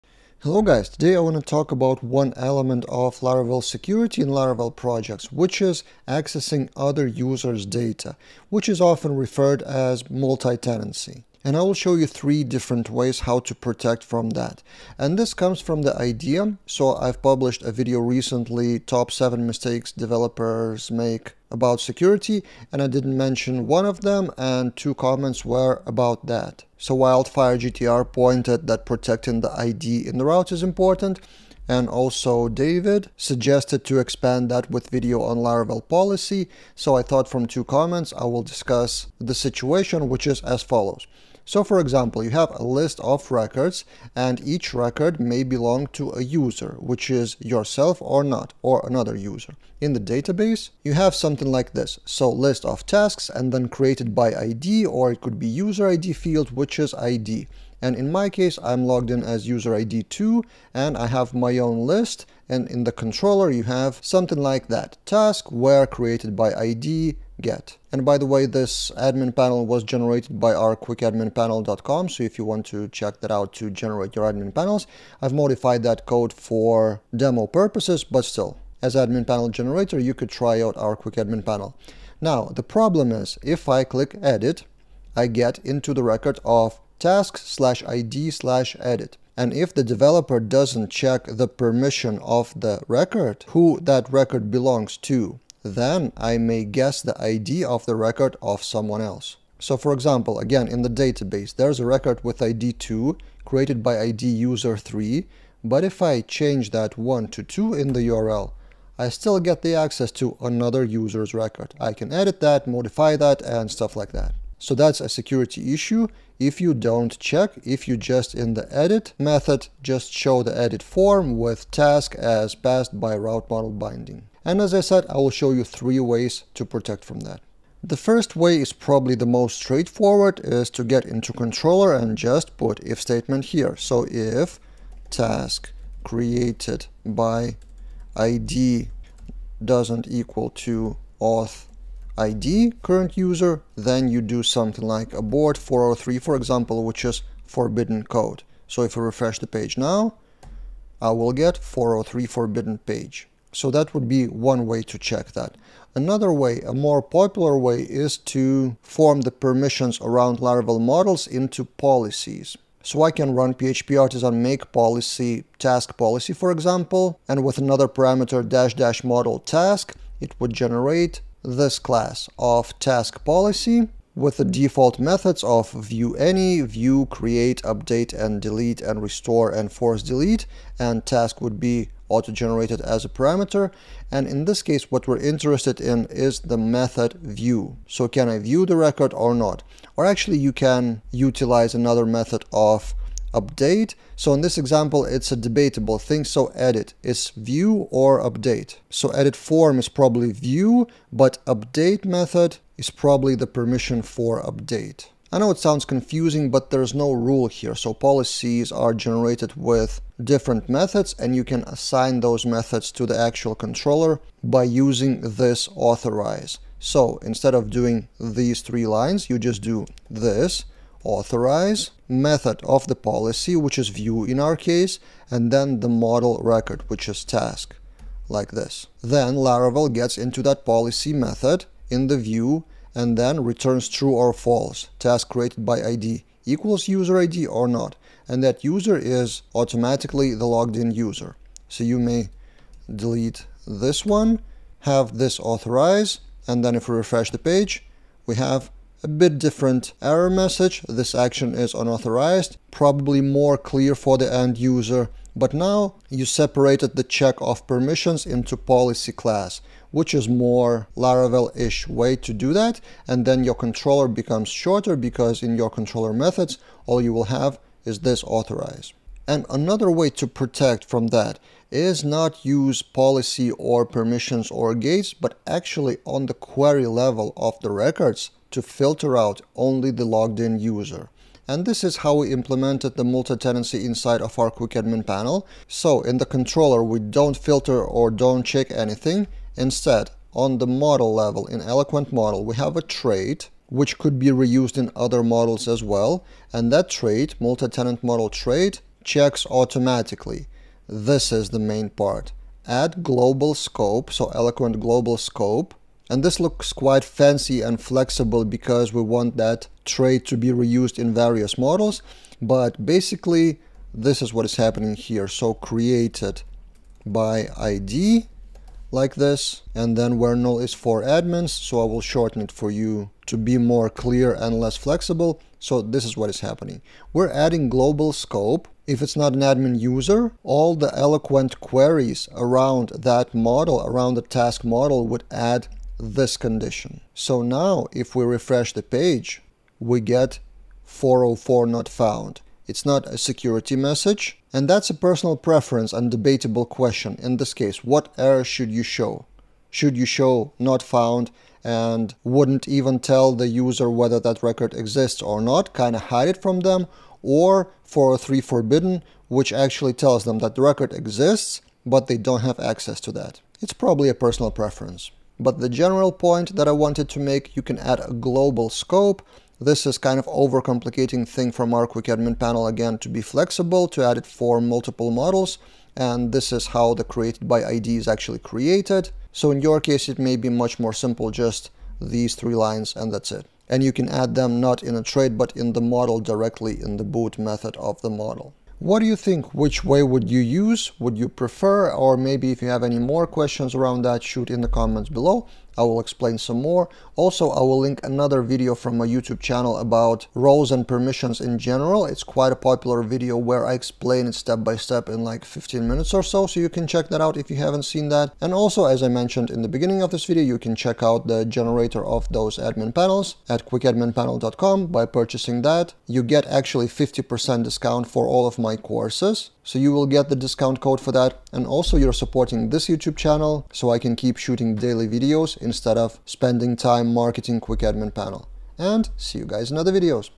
Laravel: 3 Ways to Protect Records from Access By Other Users
In this video, I will show how to prevent the user from accessing other users' records for viewing/editing, in 3 different ways.